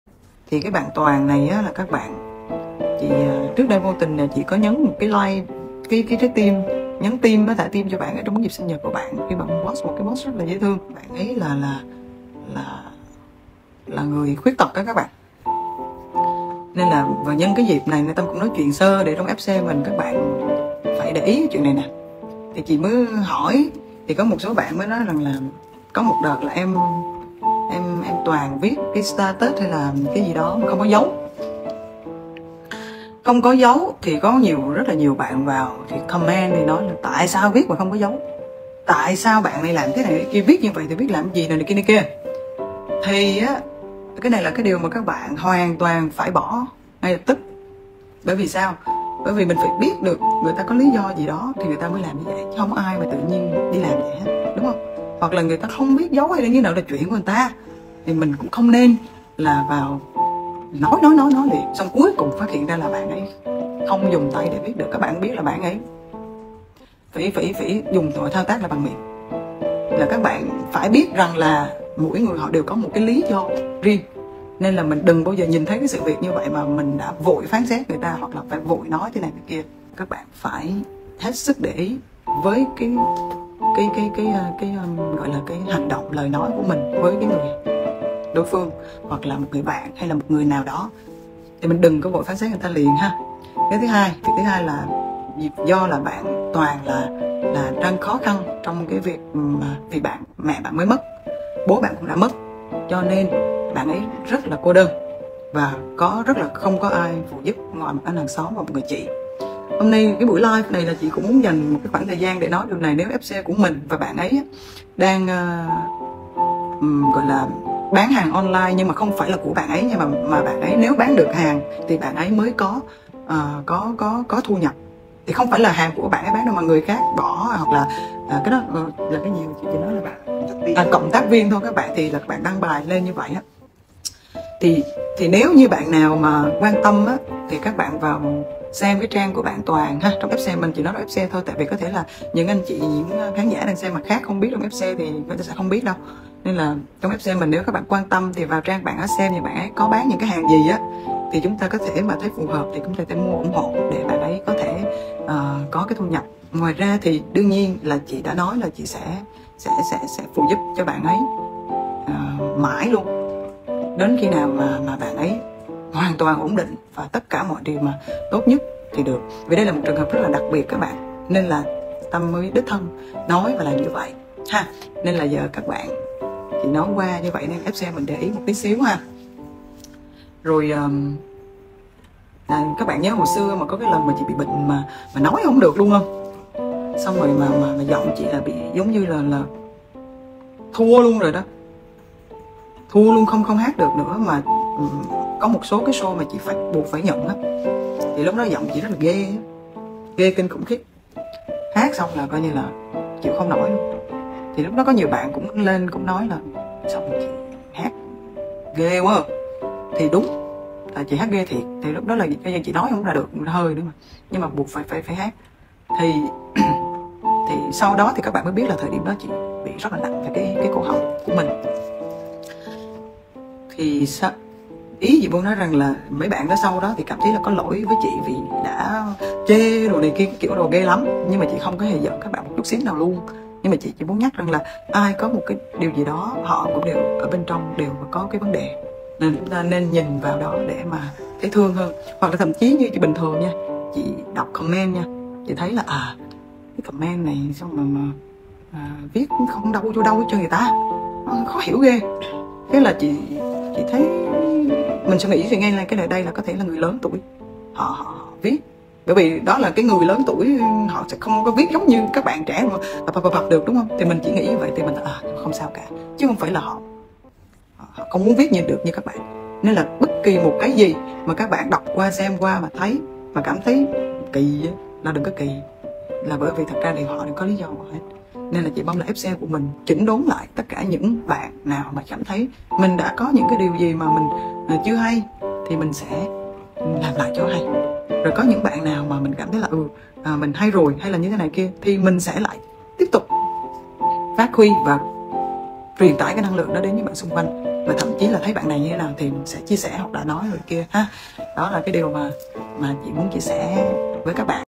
Mỹ Tâm nhắc nhở những fan “tay nhanh hơn não”
Mỹ Tâm thẳng thắn nhắc nhở những bạn fan “tay nhanh hơn não”. Đừng vội vàng phán xét người khác khi mà chưa biết rõ thực hư câu chuyện, ai cũng có lý do của họ cả! Nghe Tâm chia sẻ, fan đã rút ra được nhiều bài học, cũng không hề giận idol. Sự thẳng thắn của Mỹ Tâm cũng chính là một trong những đức tính tốt đẹp khiến khán giả thêm yêu mến Tâm.

Cre: MT Entertainment, Mỹ Tâm